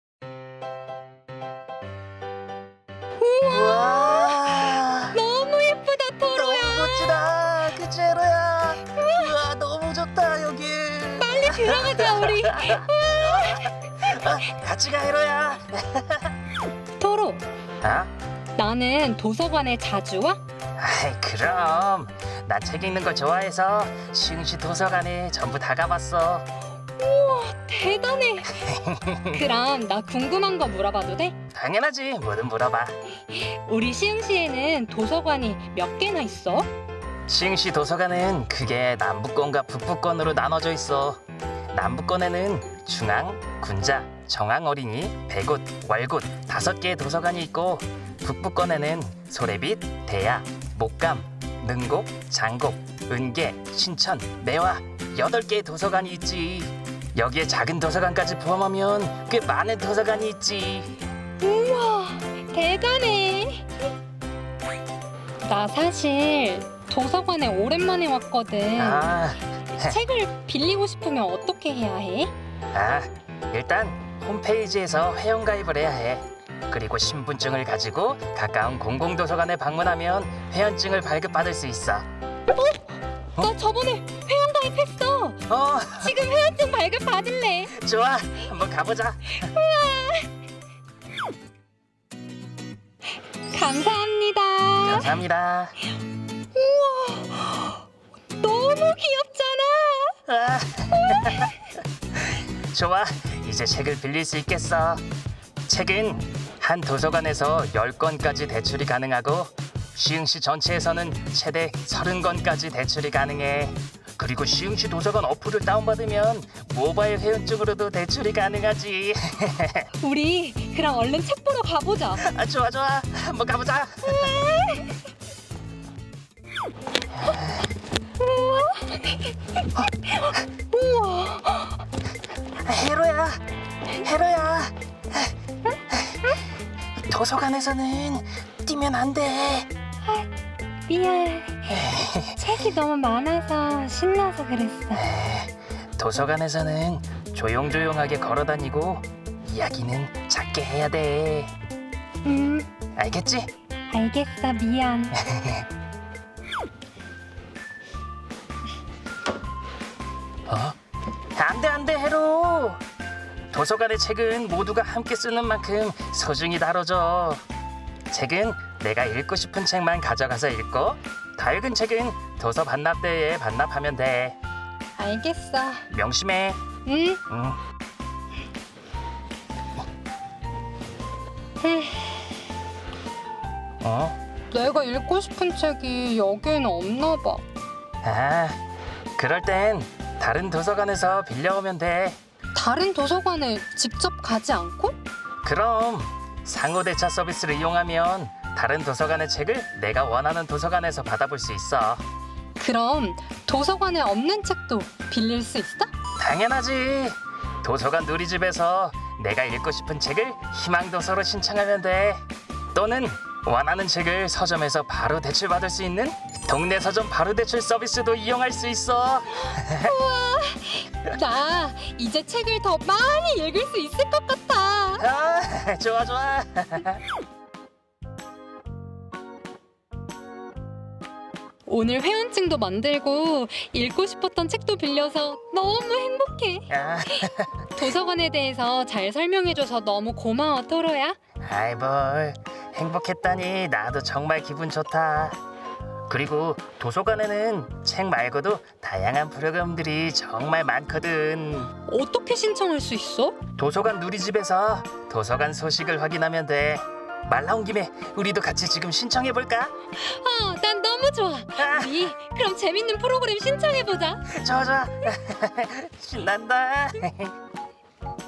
[해로토로TV] 해로토로와 함께 알아보는 도서관 이용수칙
시흥시에는 많은 도서관들이 있는데요! 📚
도서관을 이용할 때 알아야하는 수칙들이 있다고 하네요
해로토로와 같이 한 번 알아 볼까용??? 🤗